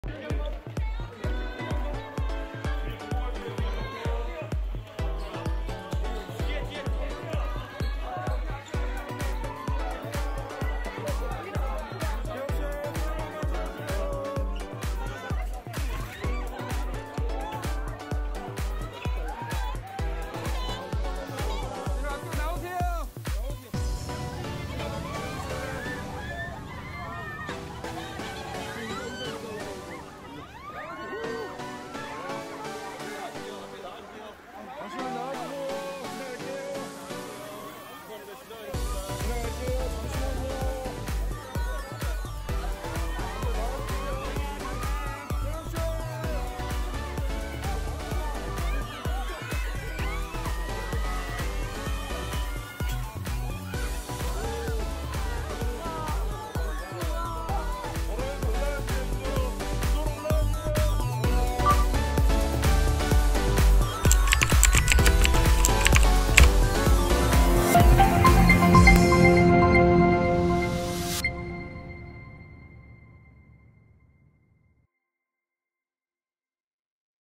「ジェニーと熱愛説」BTS・V 少年のような美しさ×笑顔であいさつ
1日早朝、男性アイドルグループBTS（防弾少年団）のメンバー、V（ヴィ）が米ニューヨークで個人のスケジュールを終えて仁川国際空港に到着、帰国した。ガールズグループBLACKPINK（ブラックピンク）のメンバー、ジェニーとの熱愛説が流れる中、報道陣に笑顔で手を振り、あいさつした。ジェニーは米ニュージャージー州のプルデンシャルセンターで行われた「MTV ビデオ・ミュージック・アワード（VMAs）2022」に出演後、8月31日に帰国している。